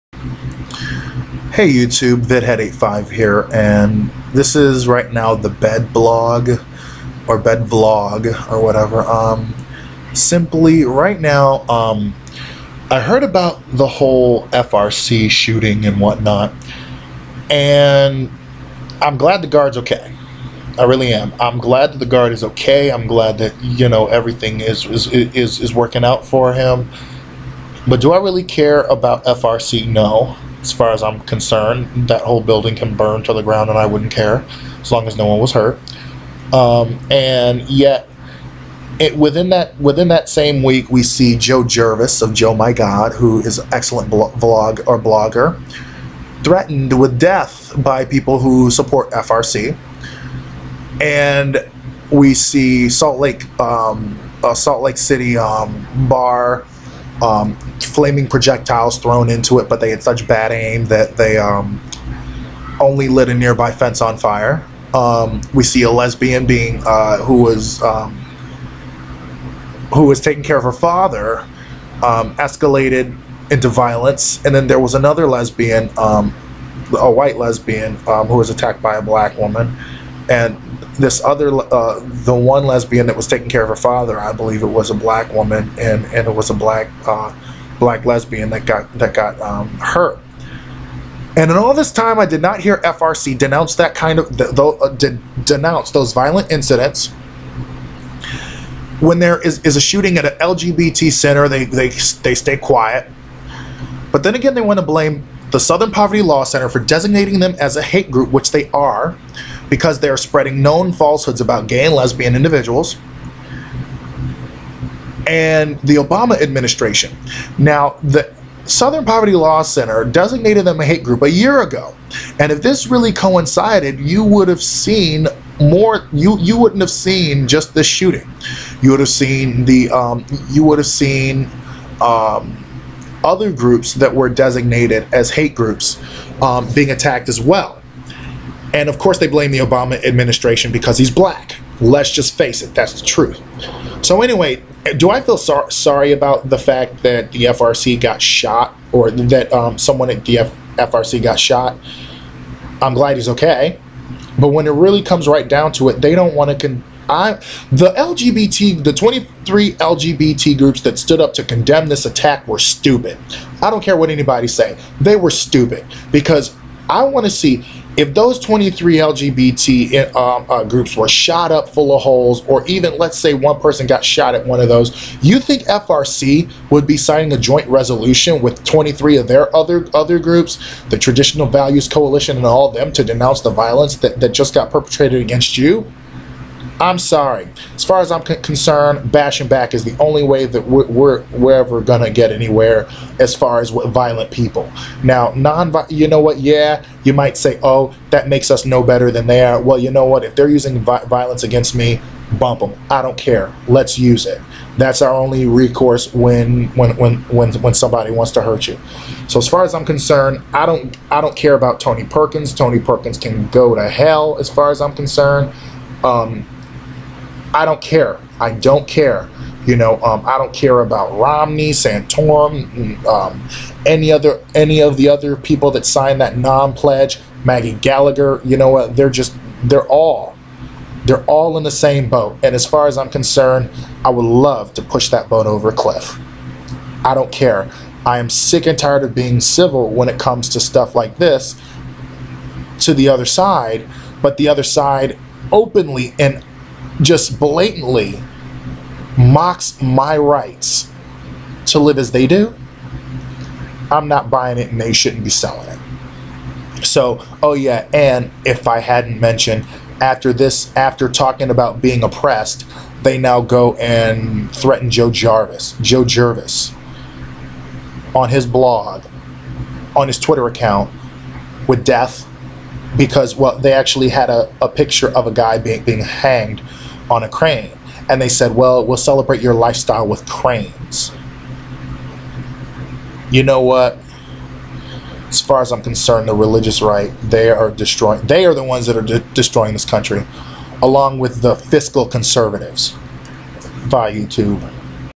FRC Shooting Incident...So What?
My thoughts on the Family Research Council HQ shooting